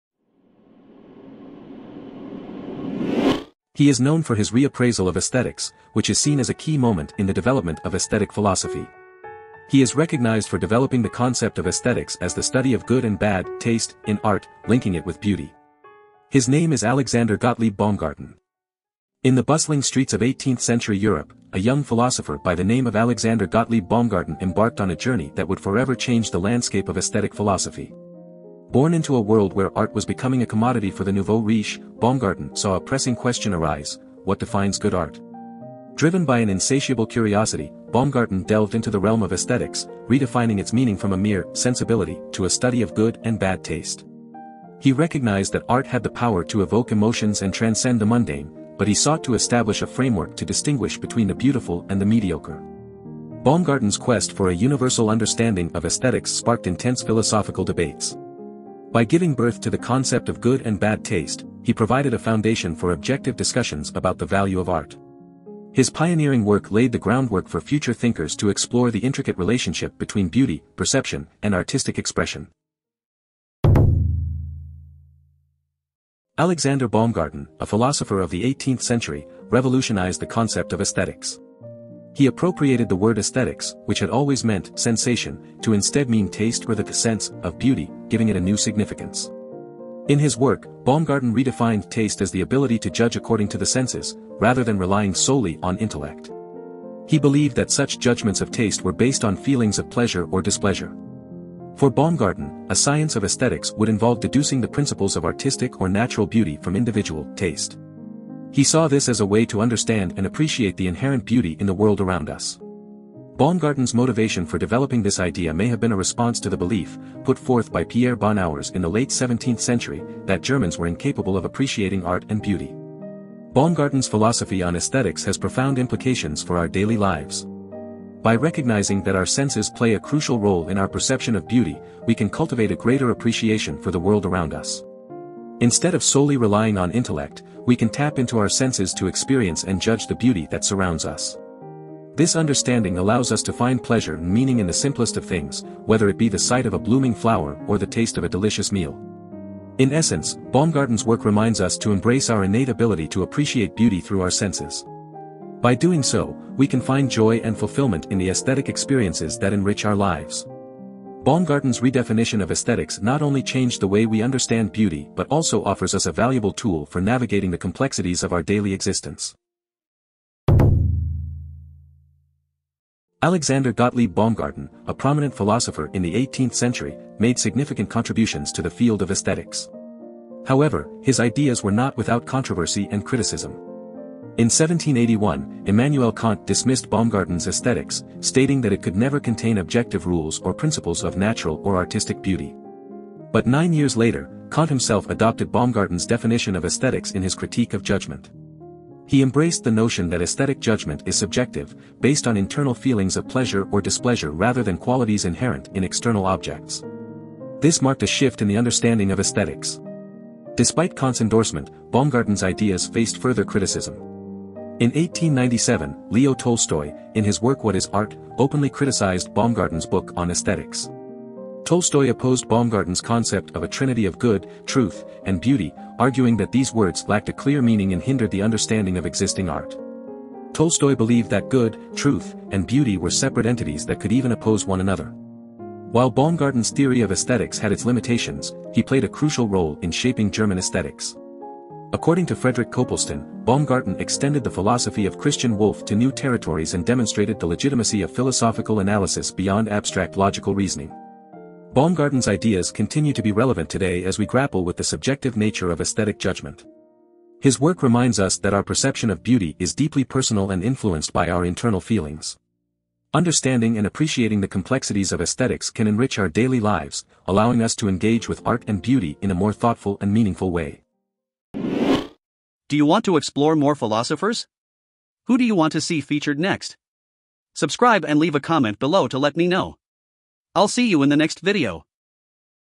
Alexander Gottlieb Baumgarten: Redefining Aesthetics｜Philosopher Biography
In the bustling streets of 18th century Europe, a young philosopher by the name of ……